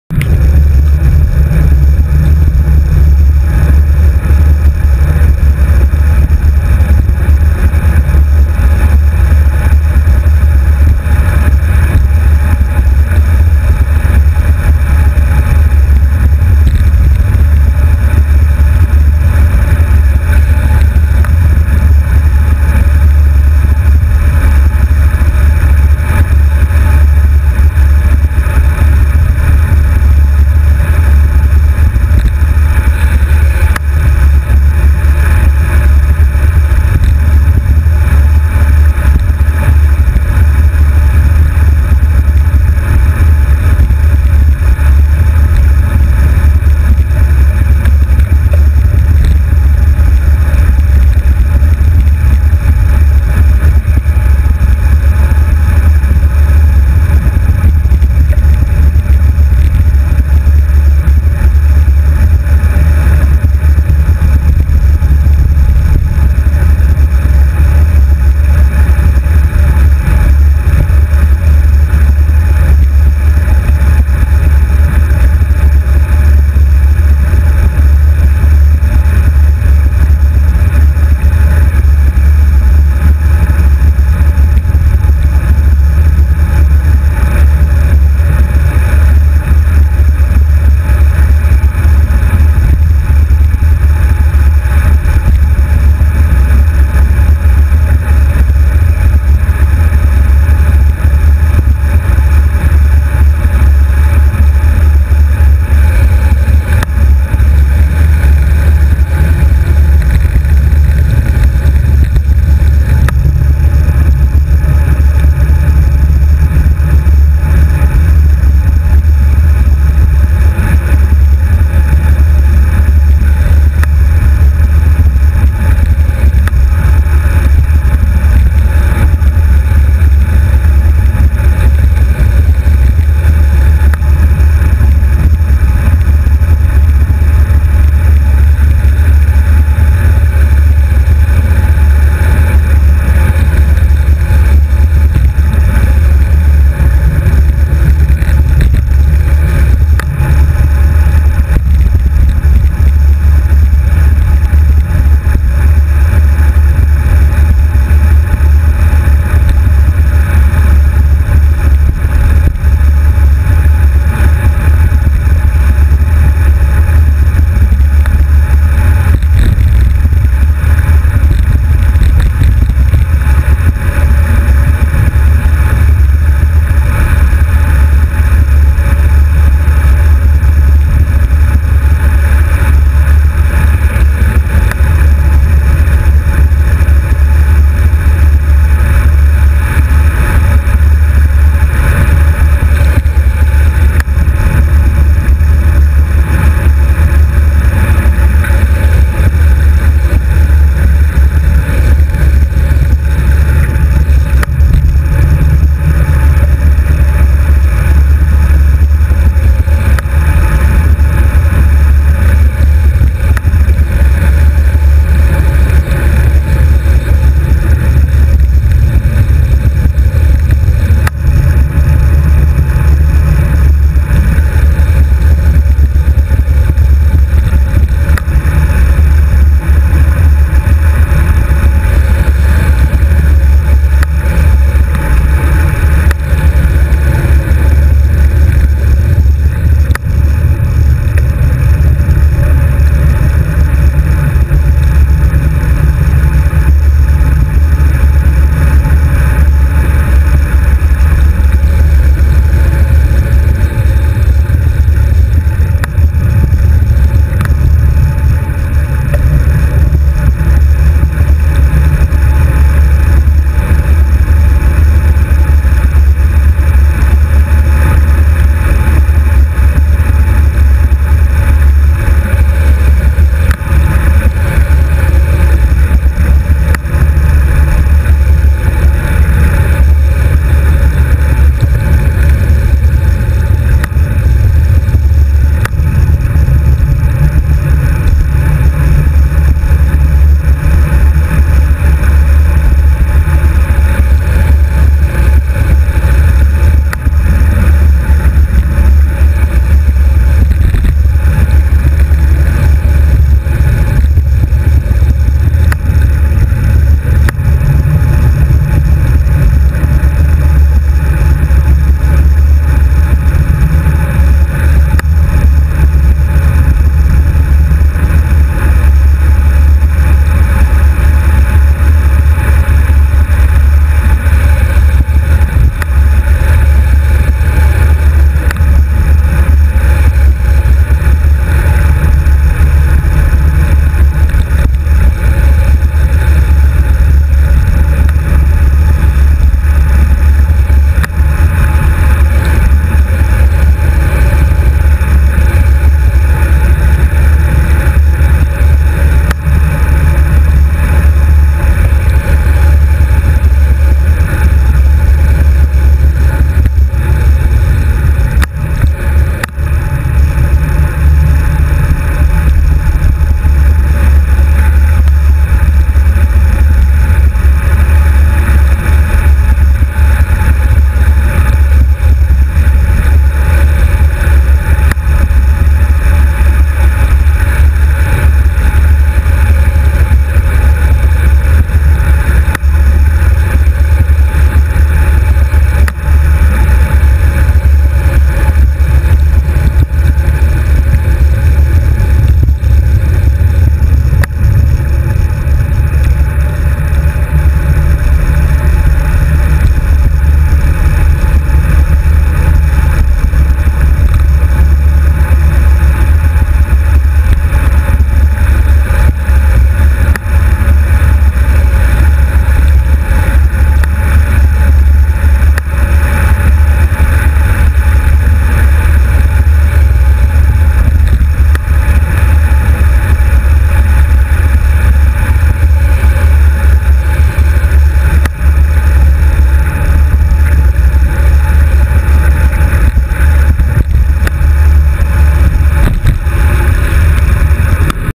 Col de Restefond 24 mai 2015
Ouverture du Col de Restefond 24 mai 2015
versant Barcelonnette